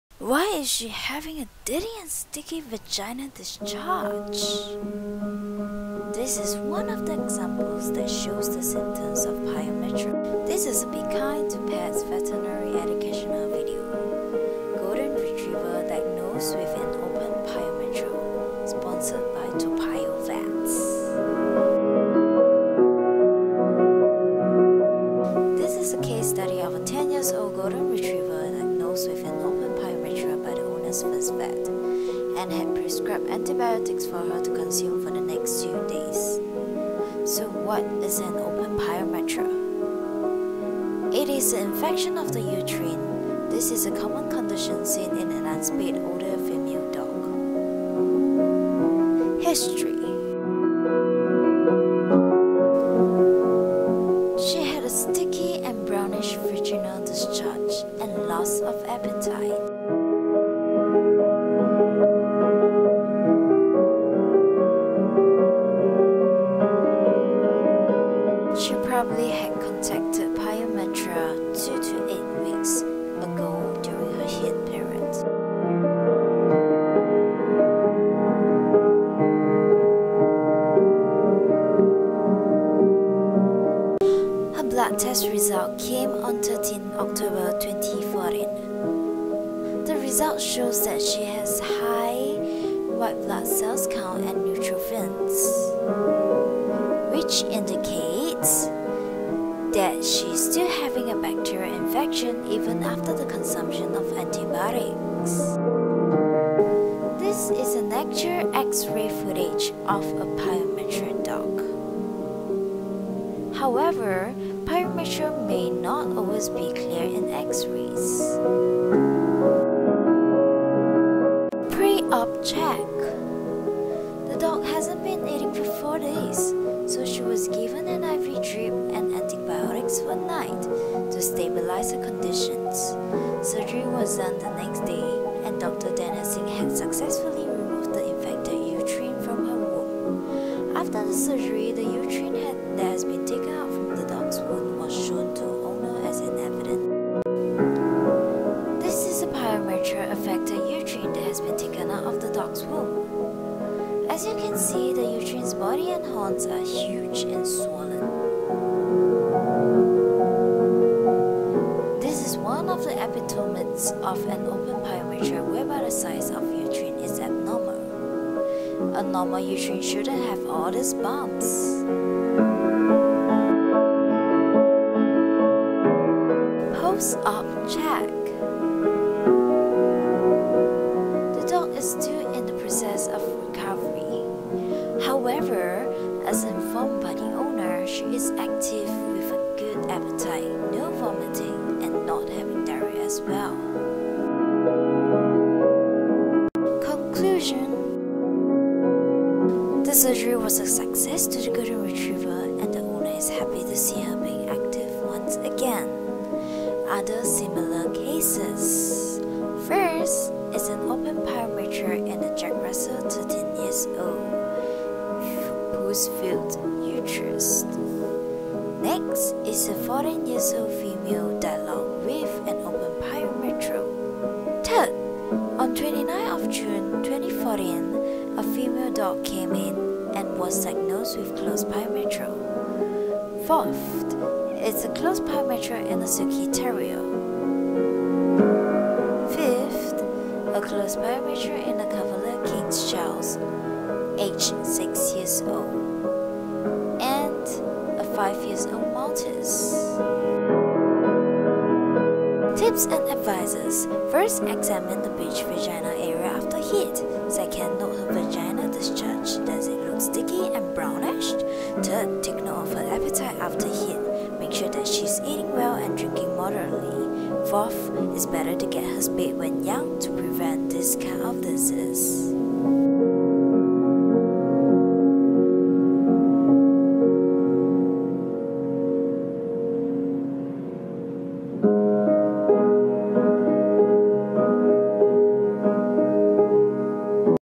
Open pyometra in a Golden Retriever
Unspayed older dogs get infection of the womb, leading to sticky and brownish vaginal discharge.This is known as open pyometra.